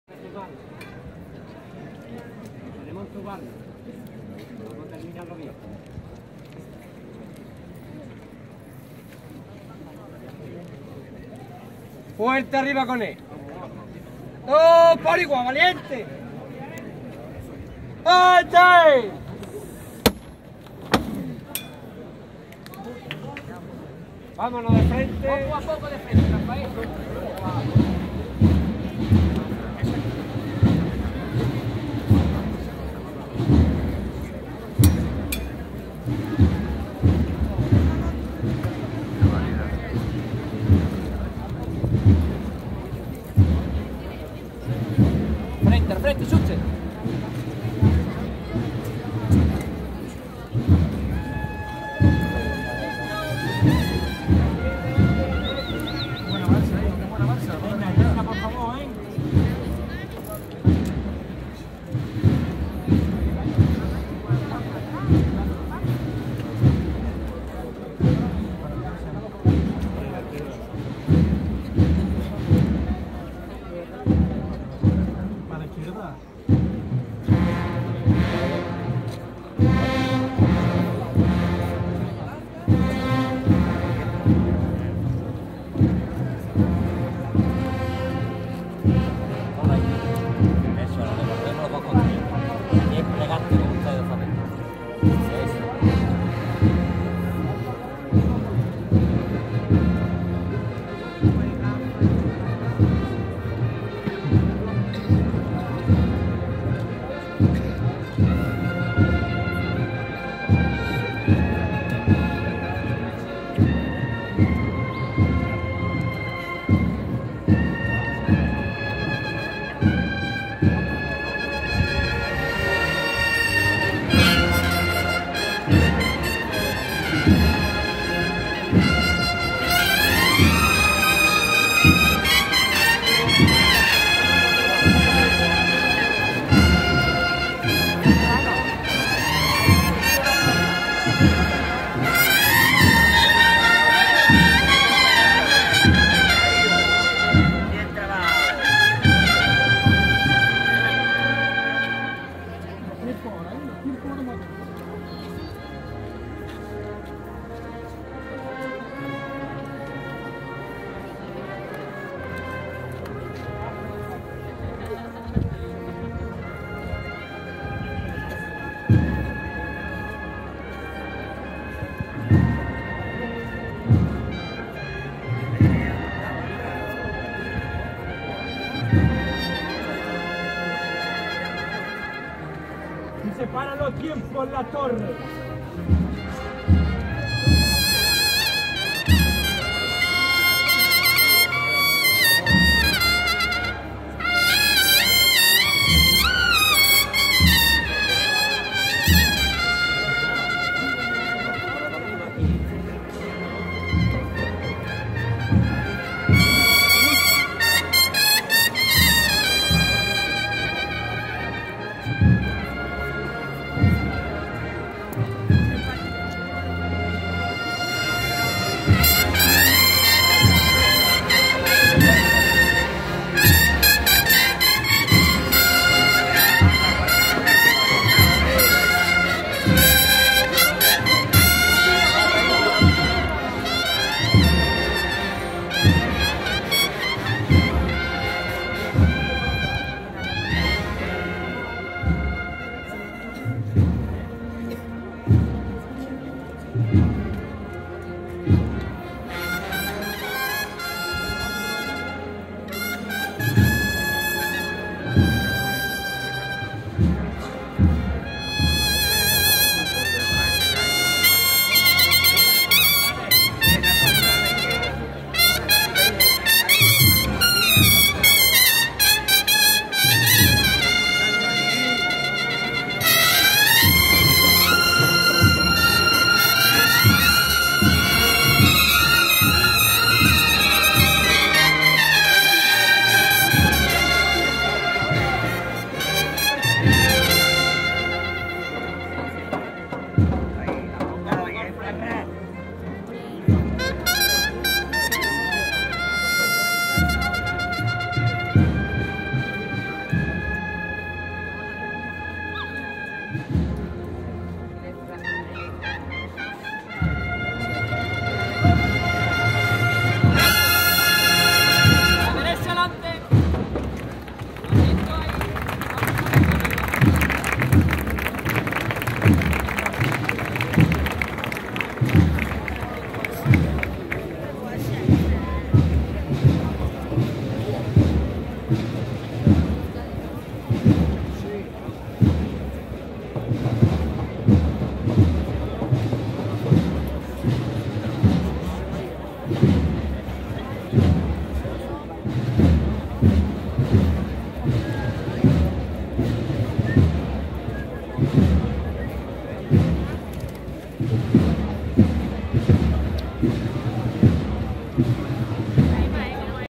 Recogida Salvación, Jerez 2018
Recogida de la Hermandad de la Salvación en su sede canónica, la Iglesia del Perpetuo Socorro, en la noche del Sábado de Pasión 24 de marzo de 2018.